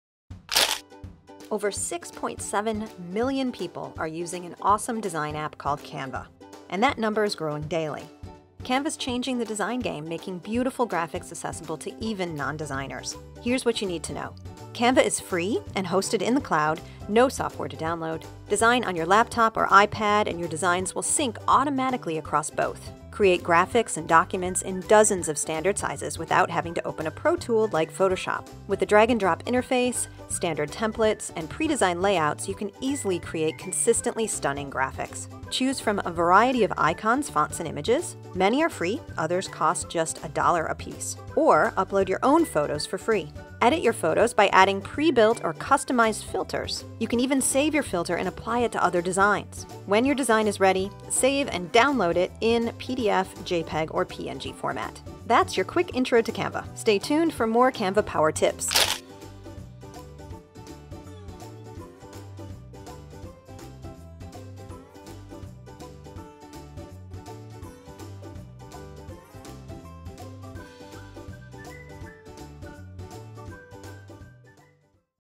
Ep2: What is Canva, and why should you use it?
Canva is the free, online design tool that's making waves in the marketing and technology space. Used by millions of people worldwide, you can make documents and graphics quickly and easily – even if you're not designer.